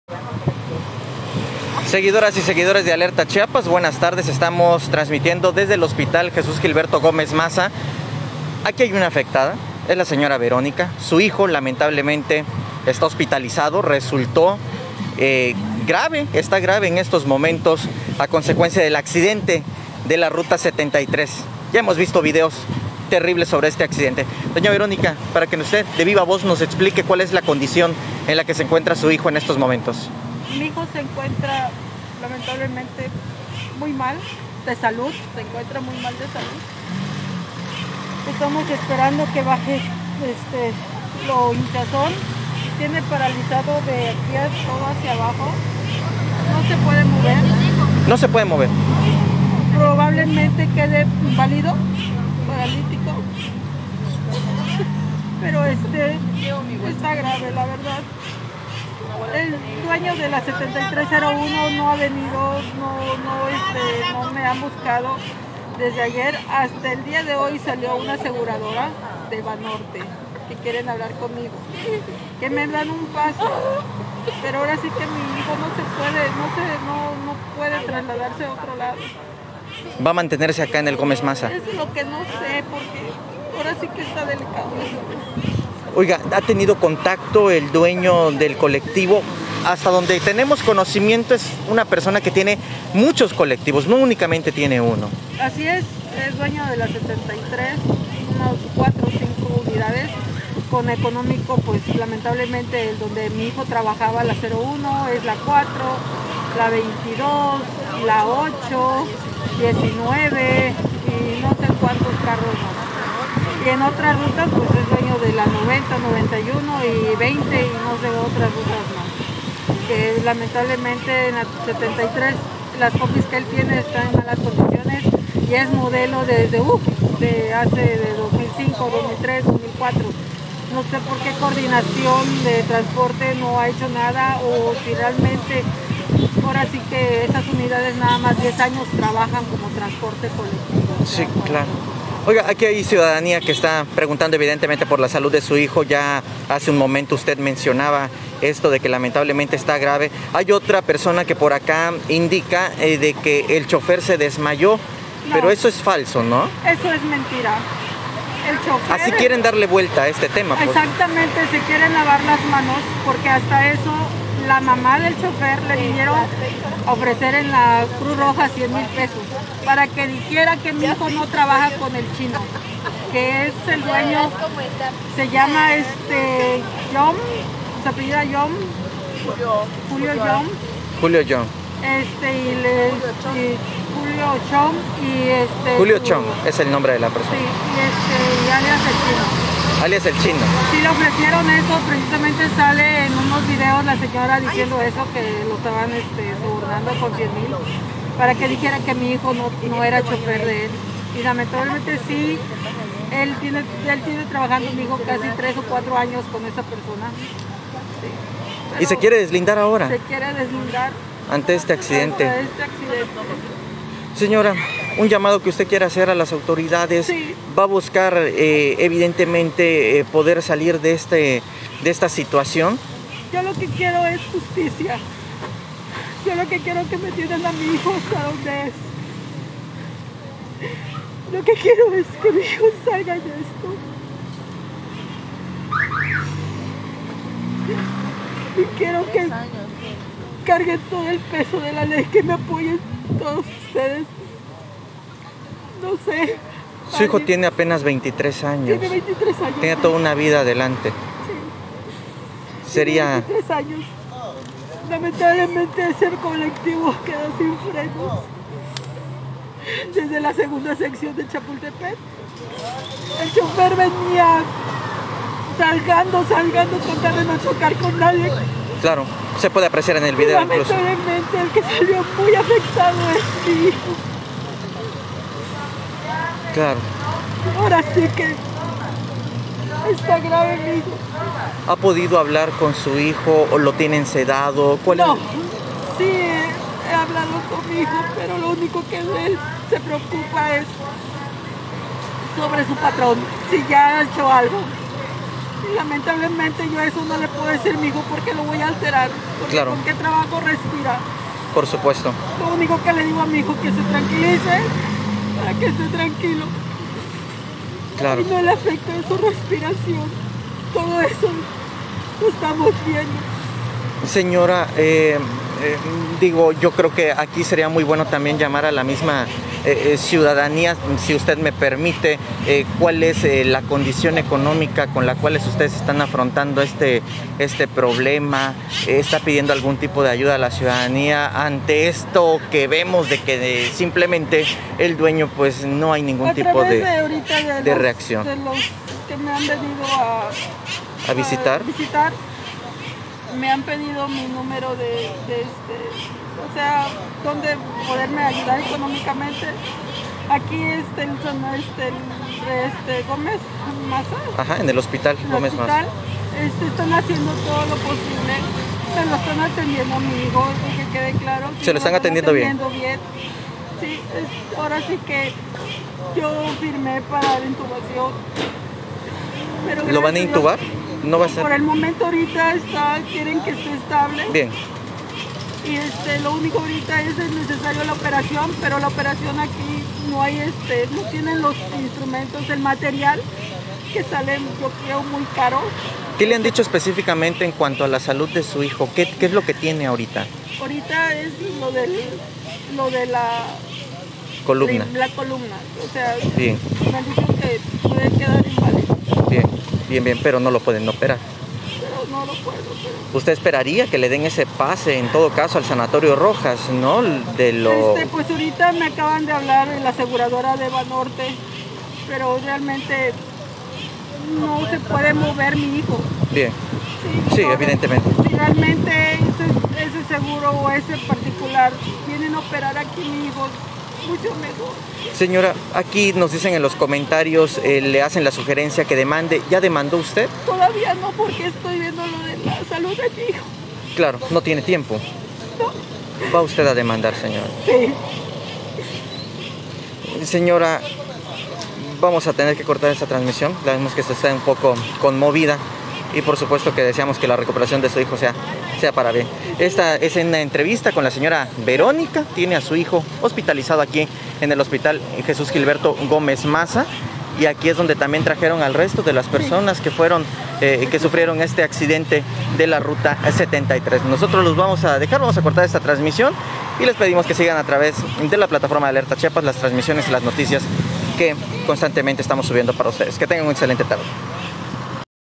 El hijo de doña Verónica está grave, venía como copiloto en el accidente de la Ruta 73
🔴 EN VIVO El hijo de doña Verónica está grave, venía como copiloto en el accidente de la Ruta 73. Exije justicia.